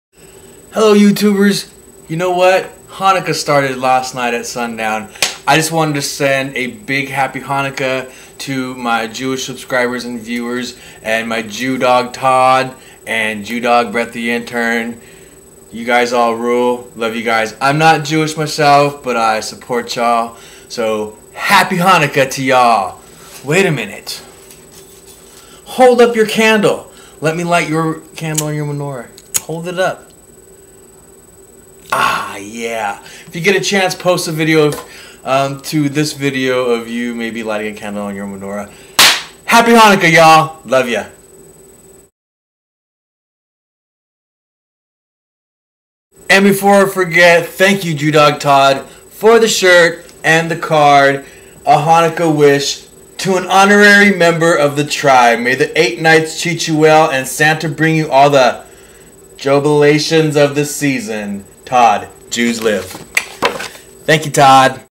Happy Chanuka (Hanukkah)
Happy Hanukkah-Chanuka to all my Jewish viewers/subscribers and friends. and thank you Jew Dog Todd for the shirt and the card